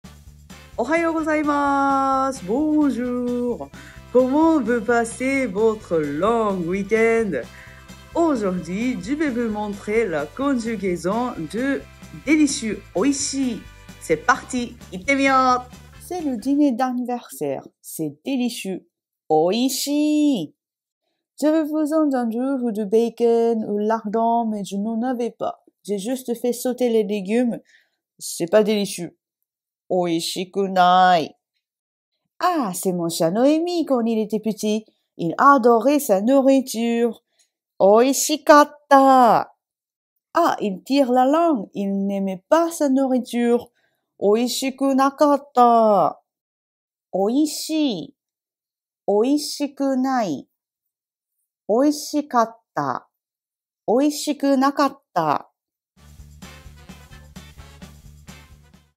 Conjugaison de l'adjectif おいしい (délicieux) en japonais
Comment vous passez votre long week-end ? Aujourd'hui, je vais vous montrer comment conjuguer l'adjectif おいしい (oishii, délicieux).
Bon week-end;)